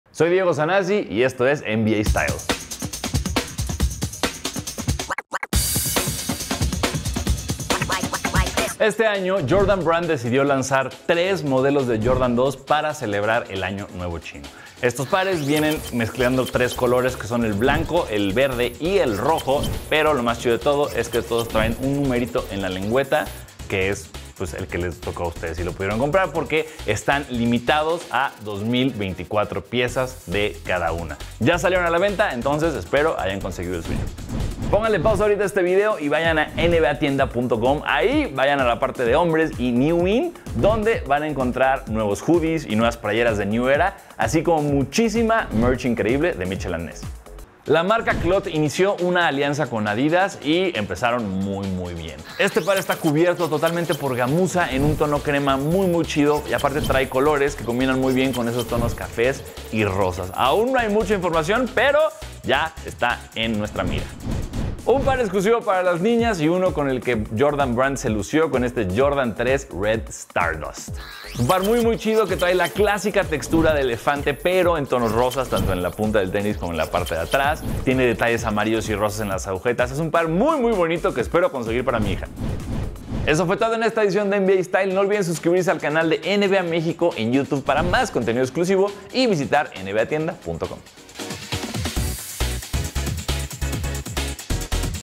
Air Jordan 2 Low para conmemorar el Año Nuevo Chino
Diego Zanassi te cuenta todos los detalles sobre el Air Jordan 2 Low, edición limitada que salieron por el Año Nuevo Chino